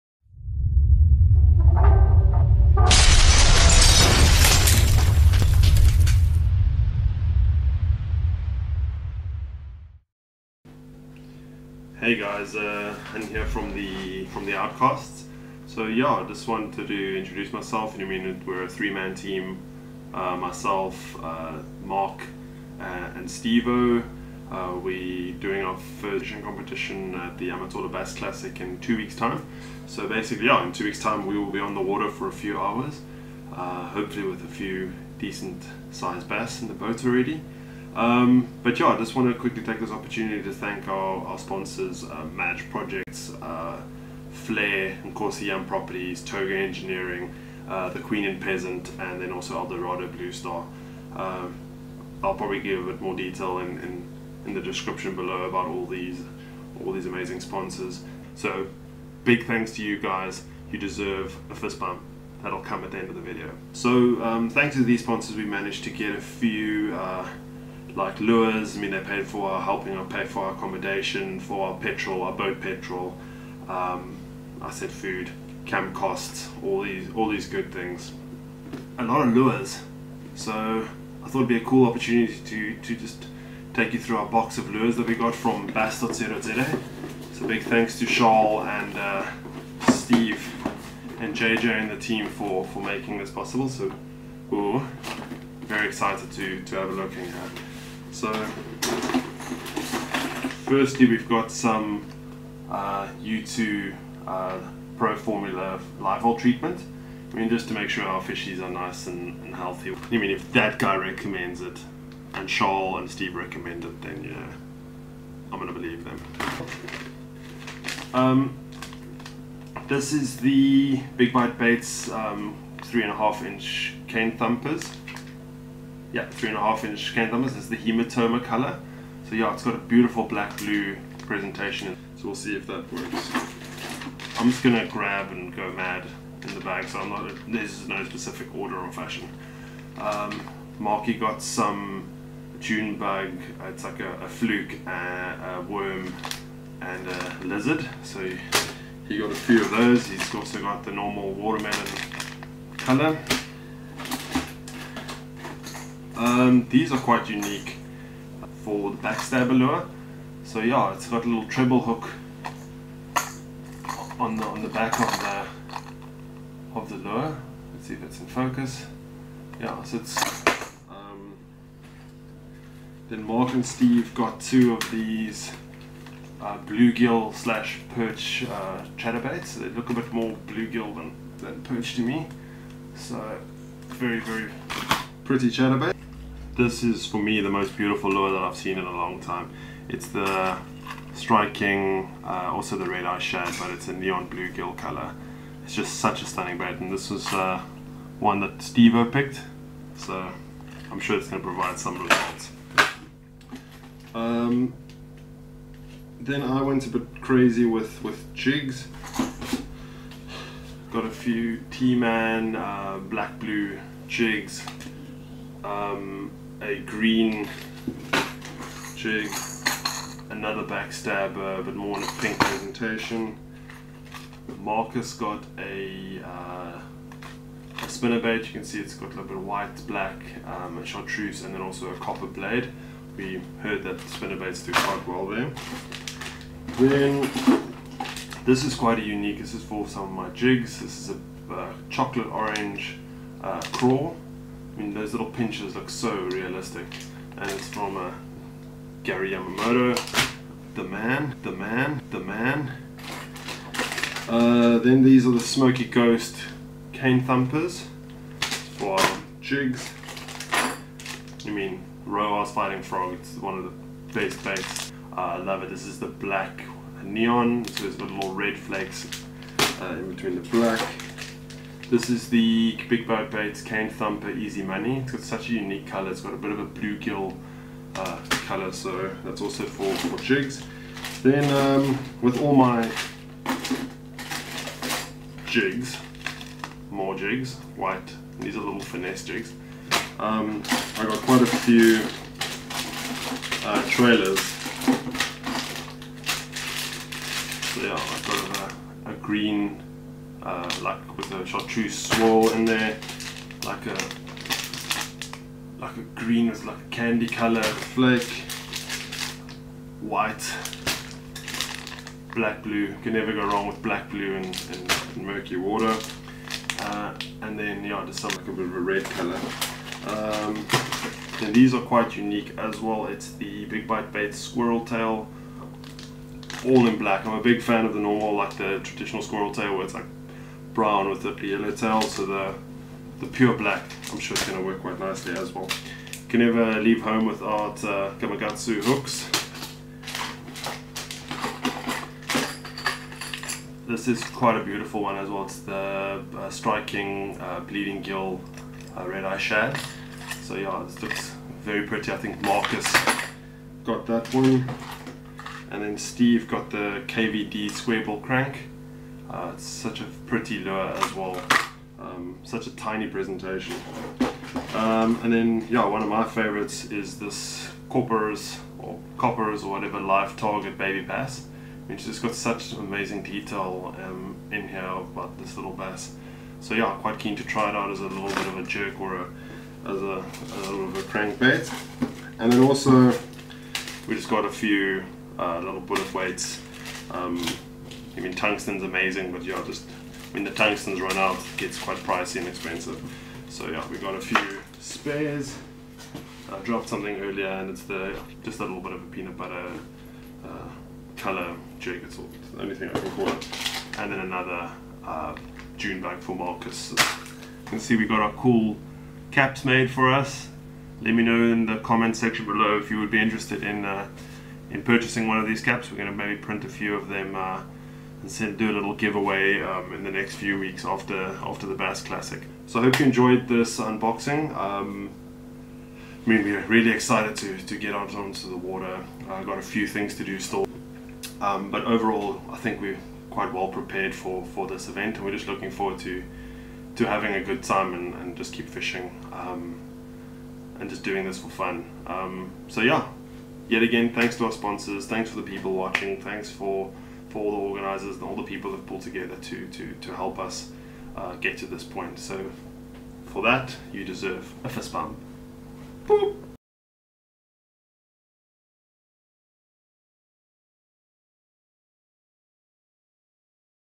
Lure Unboxing from Bass.co.za | Amatola Bass Classic'17
The Outcasts are making their fishing competition debut at the Amatolo Bass Classic in the Eastern Cape - just short of 2 weeks to go!

Thanks Bass.co.za for the speedy delivery of our lures, its always a pleasure doing business with you guys.. so I thought to say thanks I would do a unboxing video of all the lures we bought for the Bass Classic - leave a comment which one is your favorite lure that we bought?

I know I get excited when looking at a bunch of lures so i'm sure you fishing freaks like this video!

Special thanks to our amazing sponsors for making this trip a reality ...

Han, Stevo & Wilki
The Outcasts